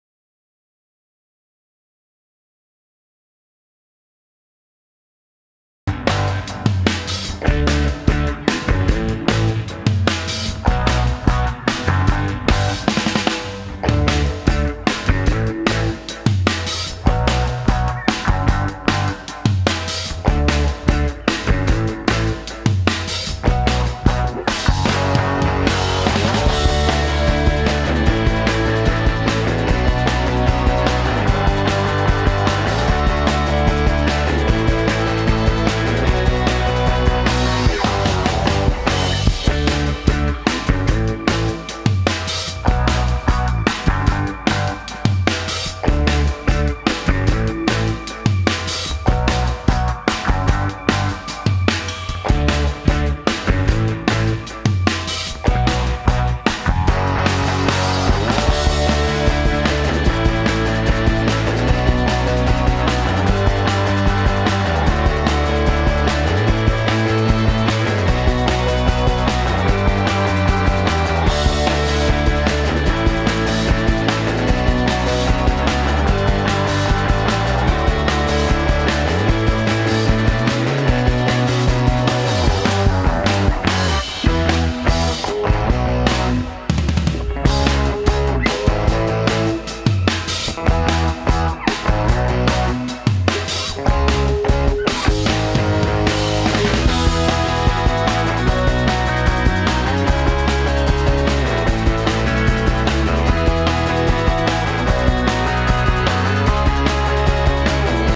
Cute Dogs Have Fun Playing Football - Clancy, Fluffy Dog Tricks Hike Ball
Move over, Puppy Bowl!  Clancy, the Fluffy Trick Dog (Keeshond) hikes a football and then has some football fun with his doggy friend for Superbowl Sunday!  Yes, NFL Scouts, Clancy is a free agent!